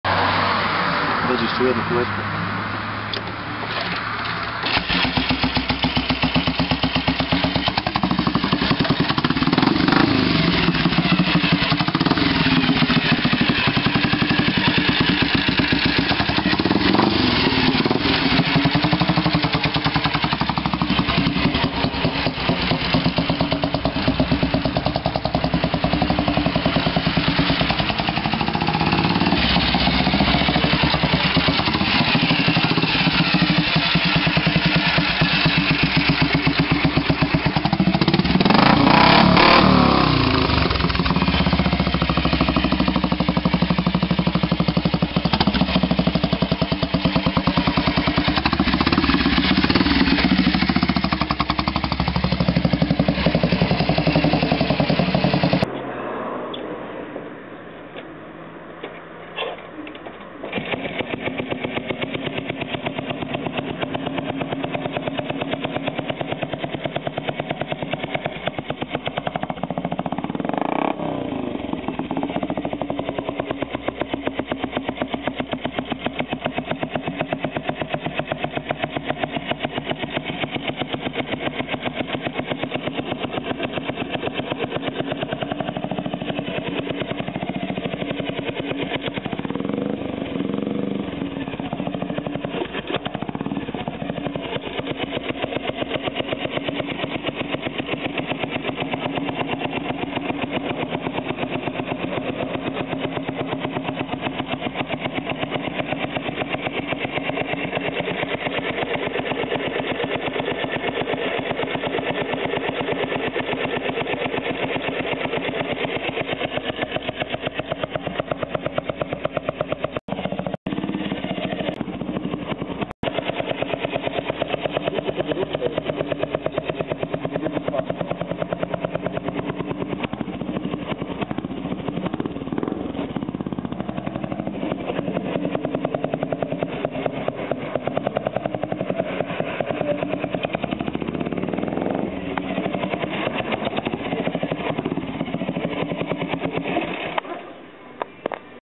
Junak M10 chopper. Staaaare lata.
Kupiłem dawno temu Junaka strasznie wieśniacko przerobionego. Uznałem, że z padliny da się jeszcze motocykl zrobić. do oryginału nie wracałem, ale po remoncie wyglądał zacnie :D